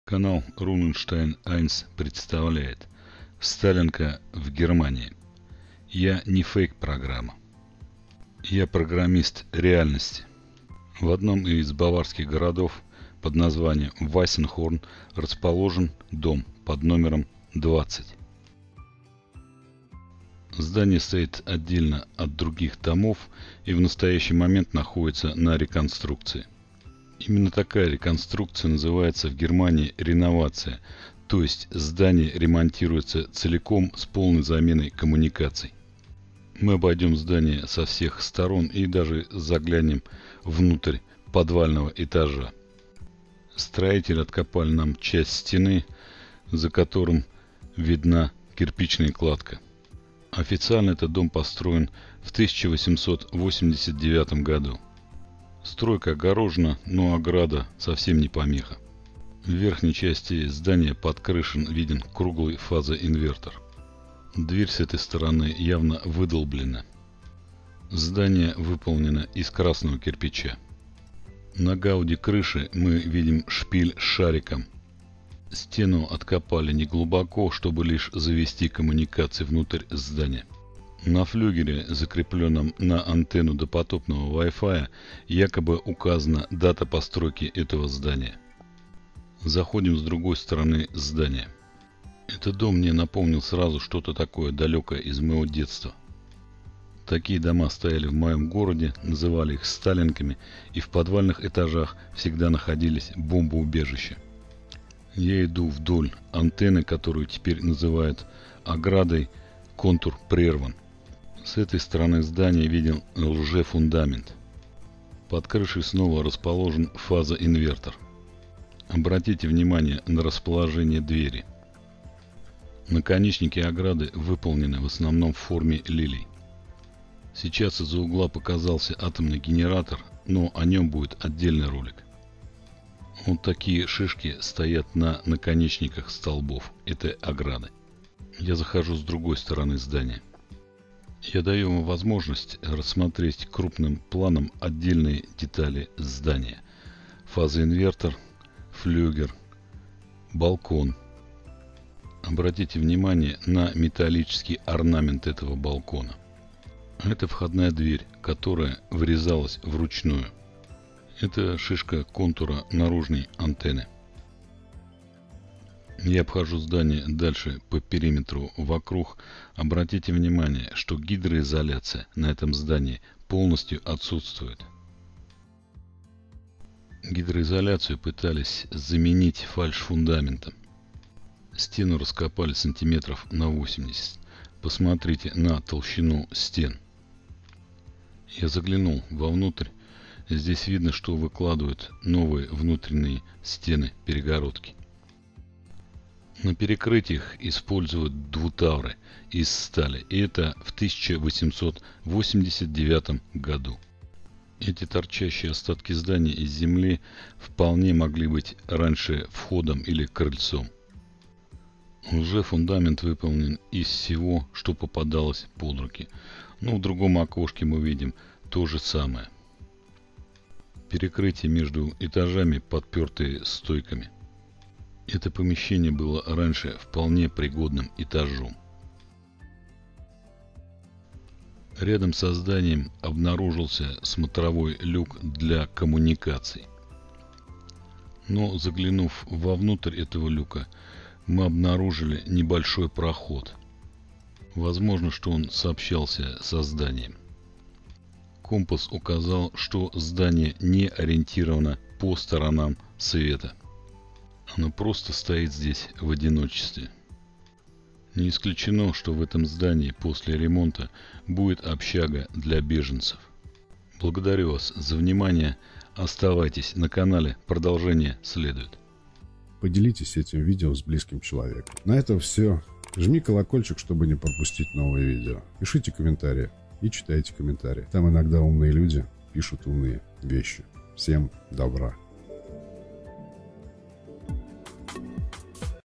"Сталинка" в Германии
Обычно мы замечаем одинаковую архитектуру по всему миру, относящуюся к готическому или колониальному стилям. Но Европа расположена не на Марсе и здесь присутствует тип зданий, которые обычно встречаются на просторах бывшего СССР. Эти дома называют "сталинками", но похожие экземпляры можно повстречать и в Германии. В Советском Союзе такие здания строили пленные немцы, а в Германии их приписали в конец 19-го века. Но "бисмарками" эти дома не называют. В этом ролике я хотел рассказать вам о том, что не только европейские стили архитектуры могут встречаться в России, но и архитектура СССР присутствует на просторах Европы.